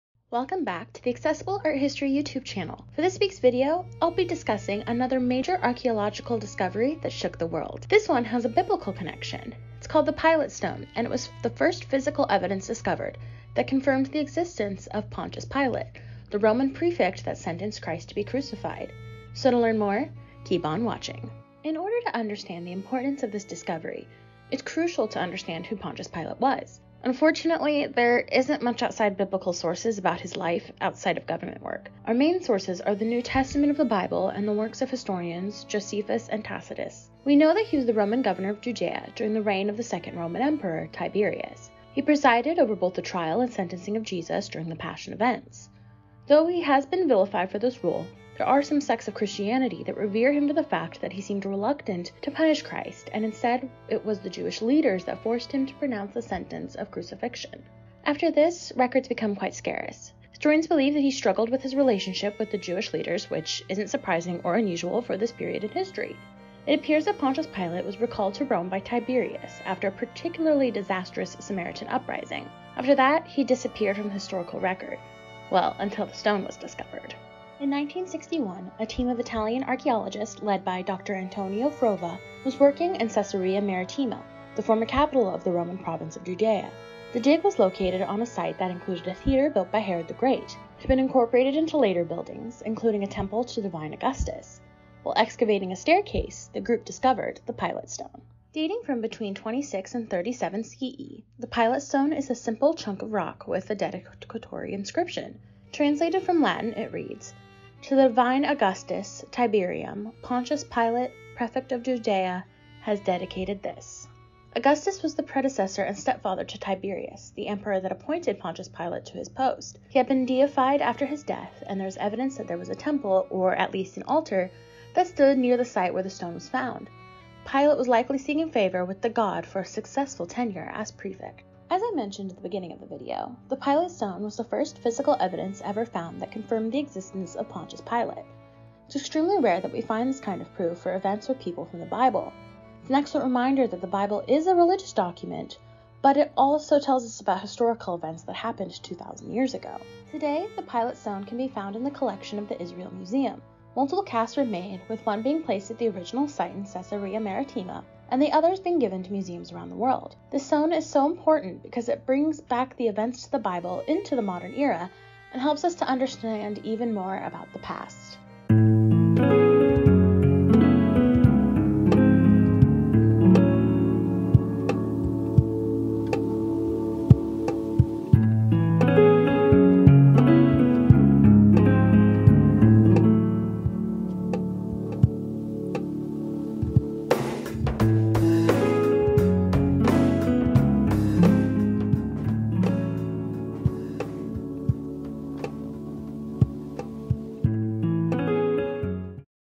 Archaeological Discoveries that Changed the World: The Pilate Stone // Art History Video
In this video, I'm discussing the history and discovery of Biblical proportions. The Pilate Stone is essential for our understanding of the events of the Crucifixion.

Sources